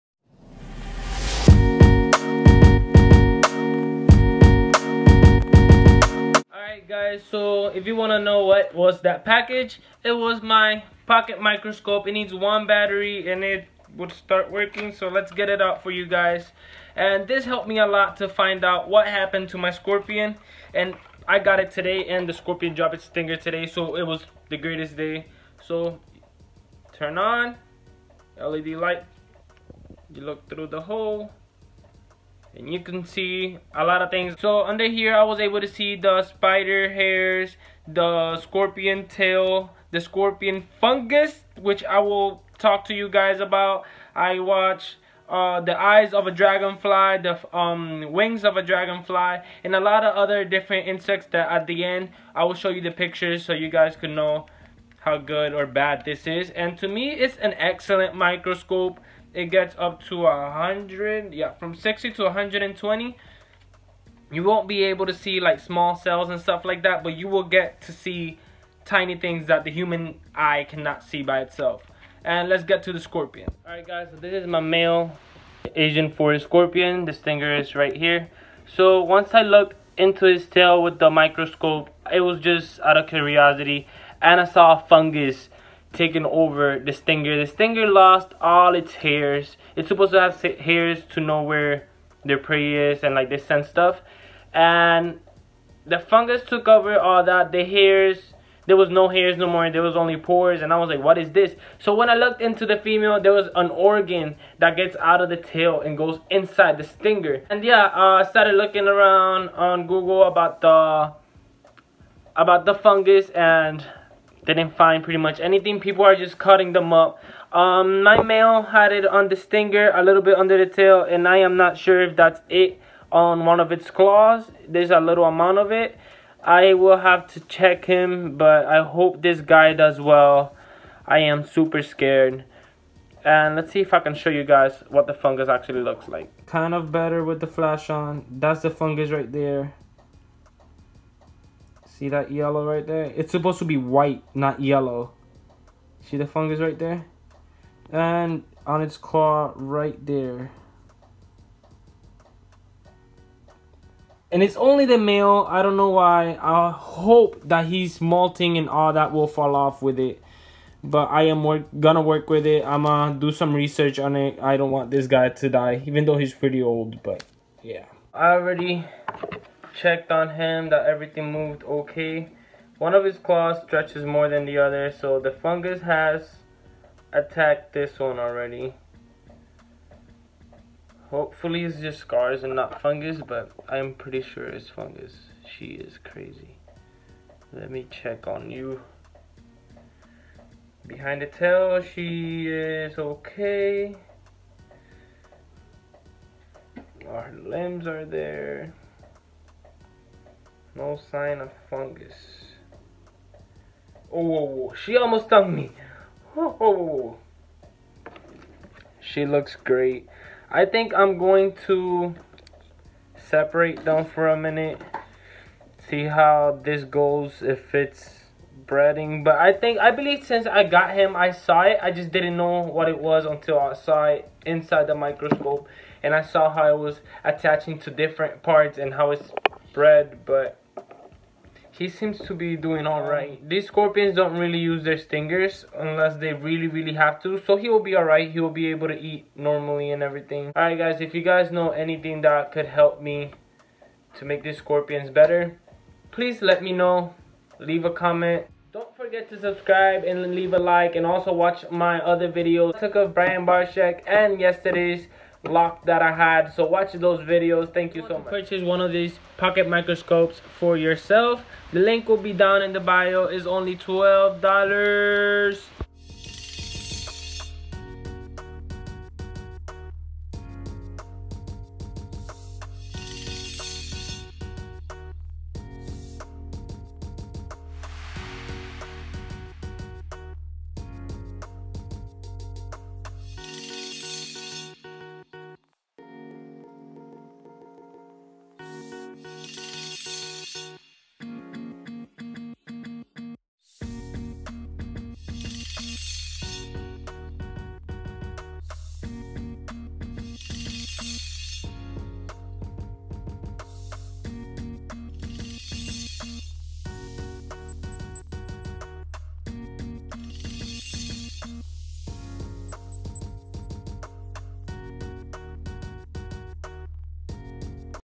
Sick Scorpion
A mystery fungus has attacked my male scorpion and broke its stinger.